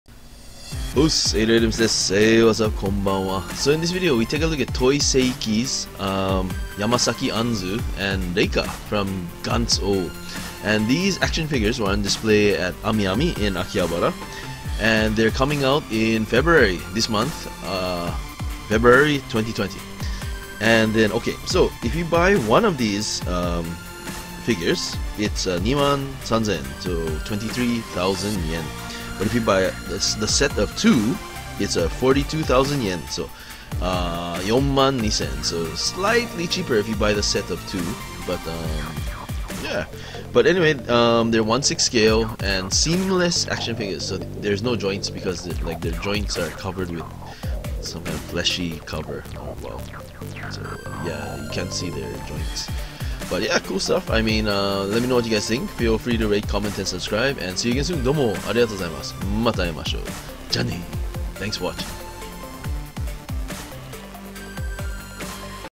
AA - Toyseiiki - Yamasaki Anzu & Reika (Gantz:O) トイセイイキ - 山咲杏 & レイカ (ガンツ:オー)
A look at Toyseiiki's Yamasaki Anzu and Reika from Gantz:O.  These seamless action figures were on display at Amiami in Akihabara.